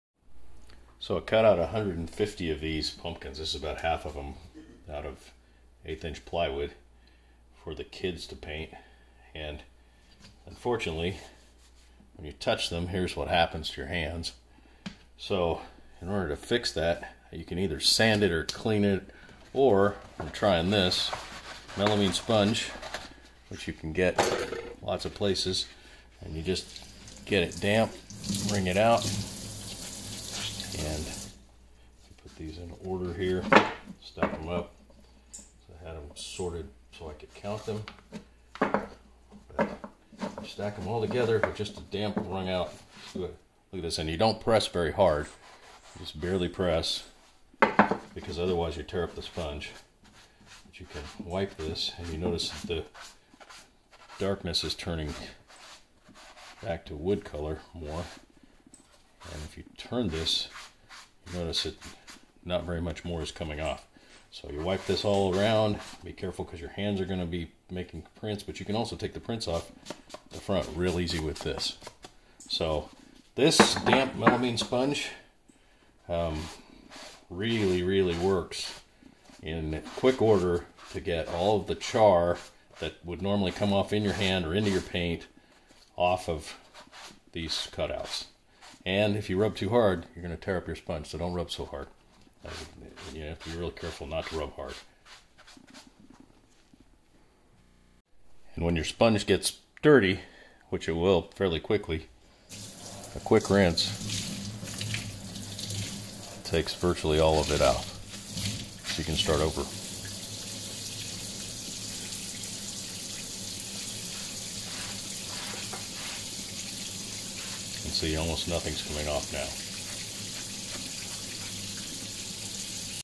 Removing Char from Laser Cutouts
I tested a melamine sponge / magic eraser to remove the burnt char from laser cut plywood.